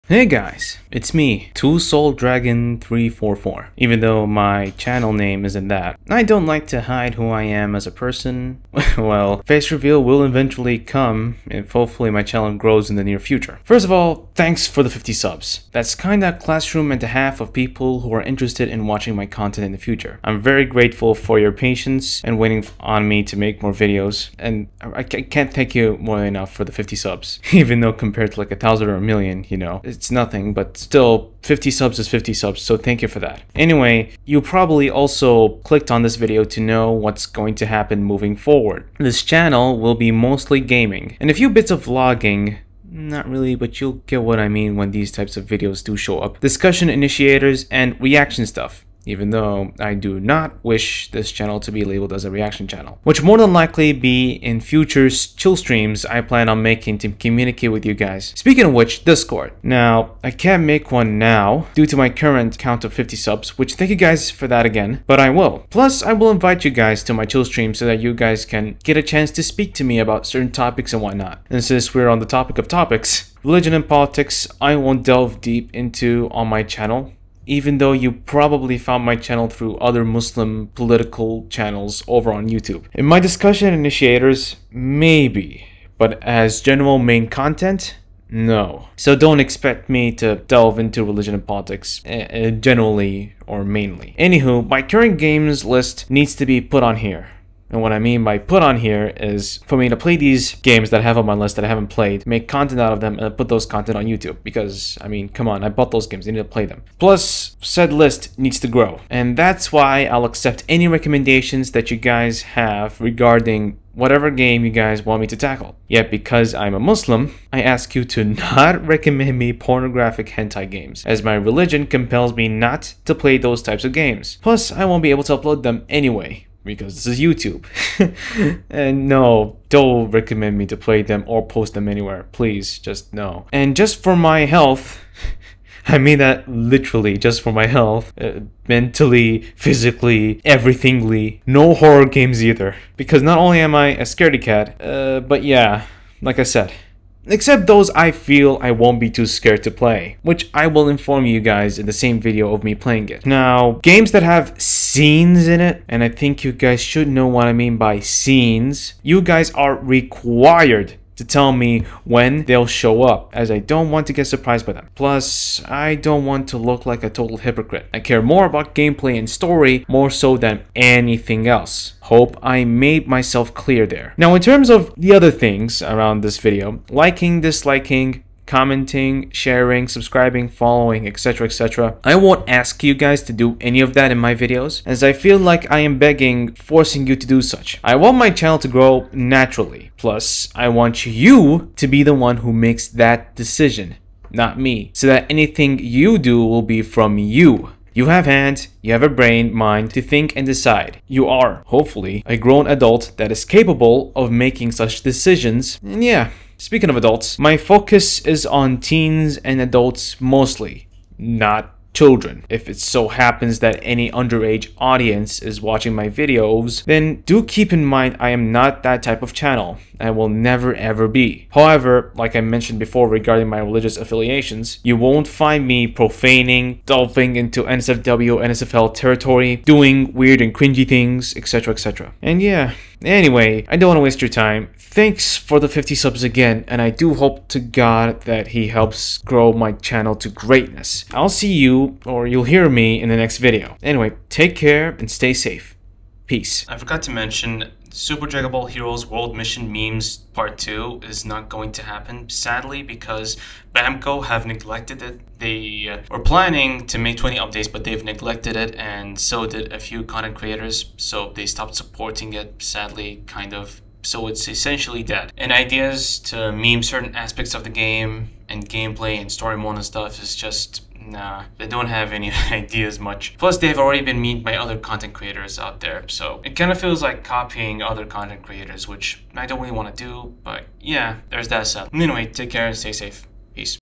Thanks for 50 subs + channel updates! [Voice Reveal]
Thank you guys so much for being there! Hope you enjoyed my content so far! More to come soon, of course, so be prepared for amazement!